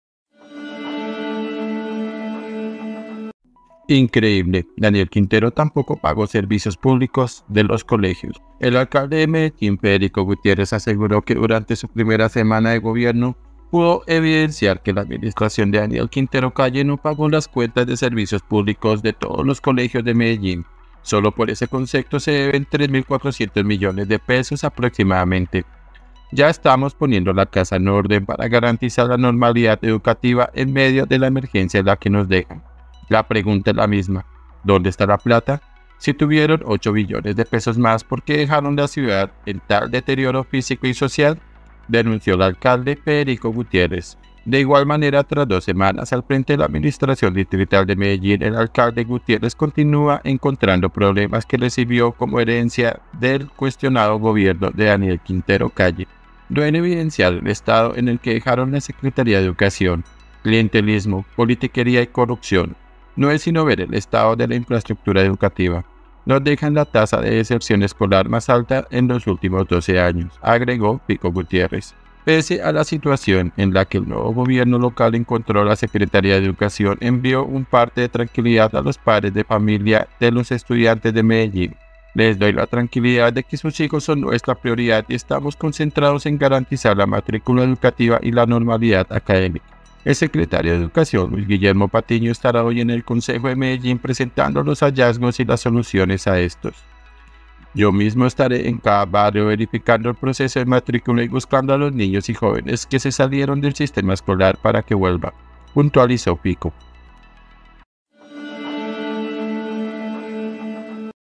INCREIBLE 🔴 DANIEL QUINTERO TAMPOCO PAGO SERVICIOS PÚBLICOS DE LOS COLEGIOS
El alcalde de Medellín, Federico Gutiérrez, aseguró que durante su primera semana de gobierno pudo evidenciar que la administración de Daniel Quintero Calle no pagó las cuentas de servicios públicos de todos los colegios de Medellín.
IFM NOTICIAS